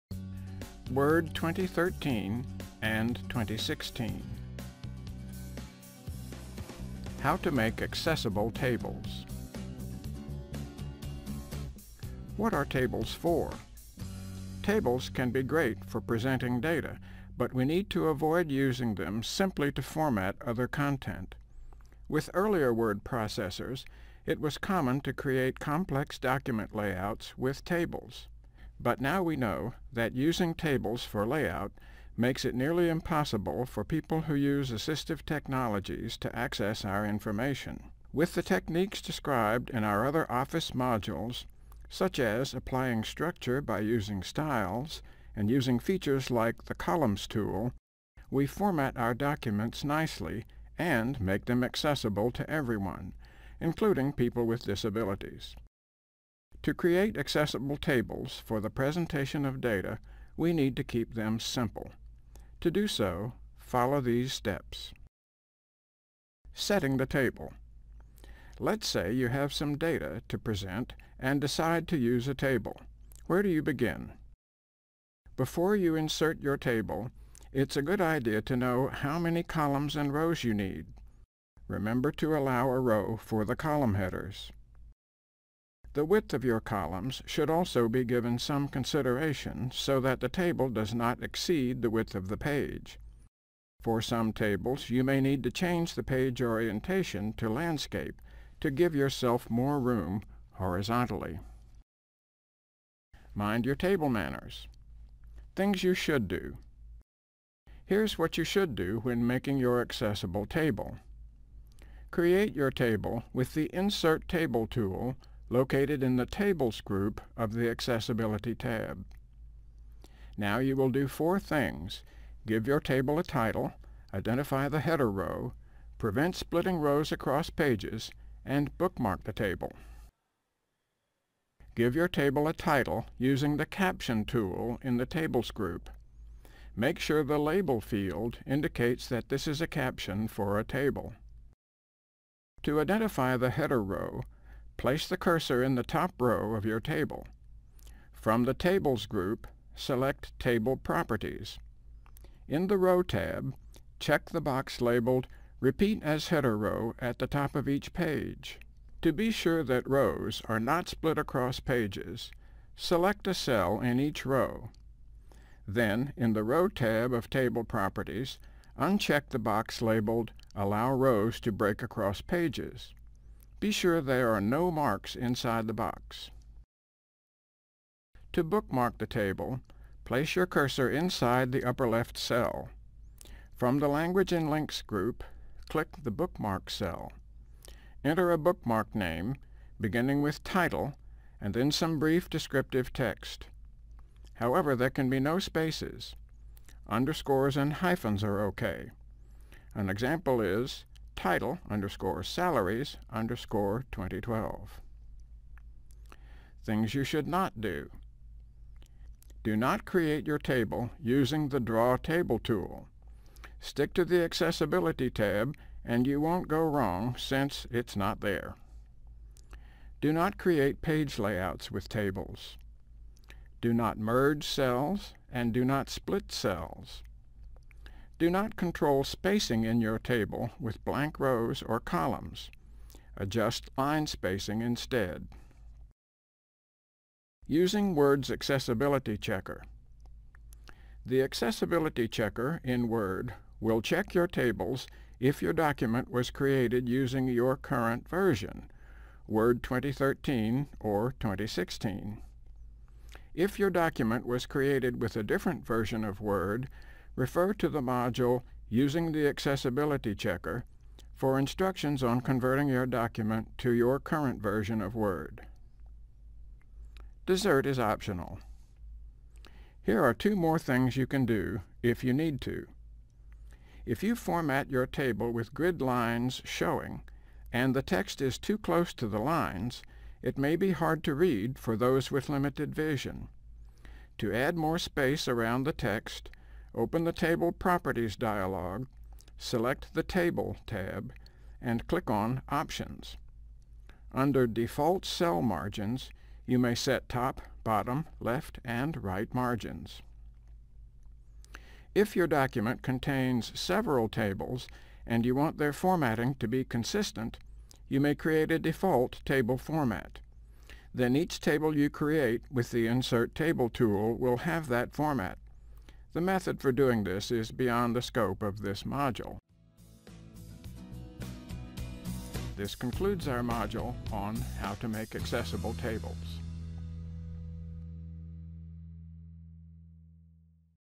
Word 2013 & 2016: How to Make Accessible Tables
To create accessible tables for the presentation of data, we need to keep them simple. This module teaches you how to create and edit tables in Word using the correct tools so they are accessible to all.